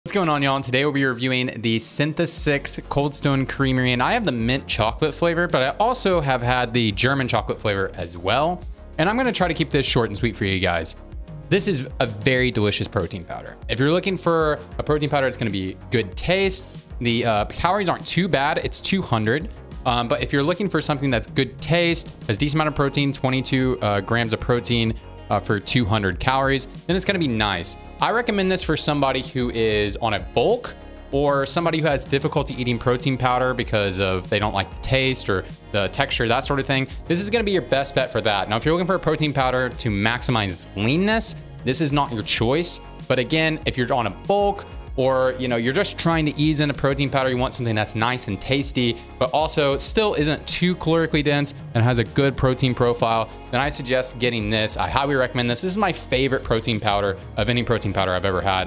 Tastiest Protein Powder? Cold Stone Creamery!
In this video I am reviewing the Mint Chocolate and German Chocolate protein powder from BSN Syntha-6 and Cold Stone Creamery!